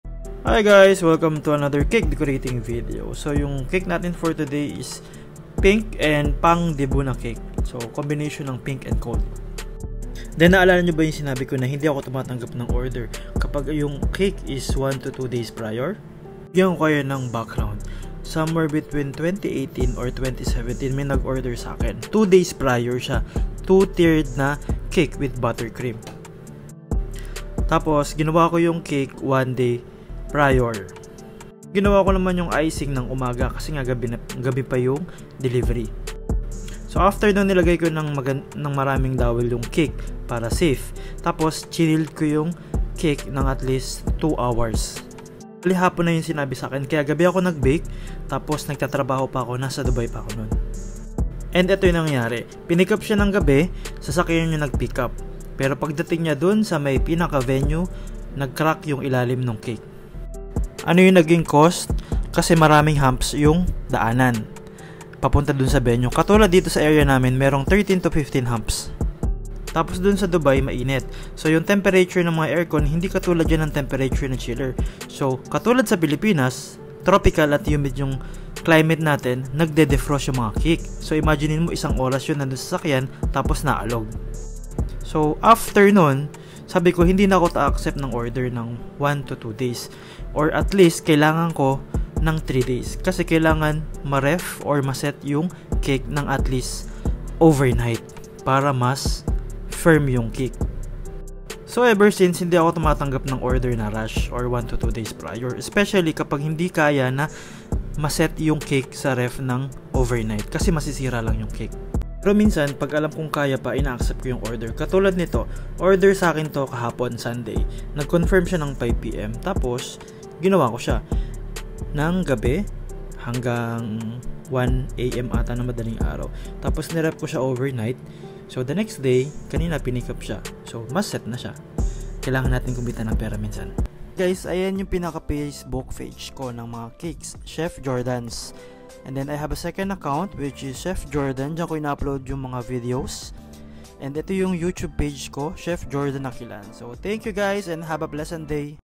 Pink and Gold Theme Debut Cake
Paano nga ba mag price ng cake?
 Bakit iba ang price ng cake pag Homebaker vs. sa Cakeshop sa MALL vs. sa high end Cake shop?

FB: Chefjhordan (with 30,000+ followers, meron kasi Dummy account posting Indian Videos-hindi yun)